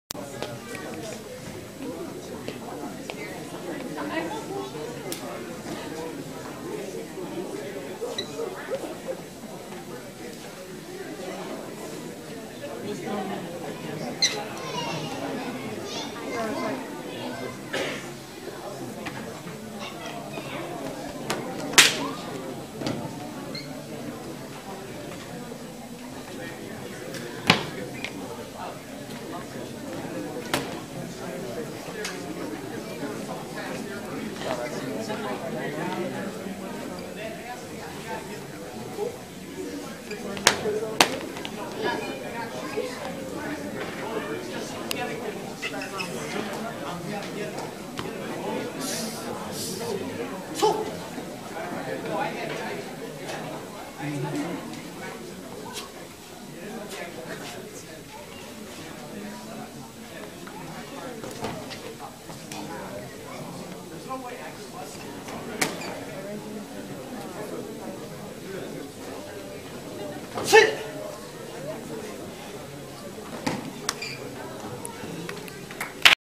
Shaolin Snake Style Fist - 少林蛇形拳
Southern Hung five-animals style of a snake fist form.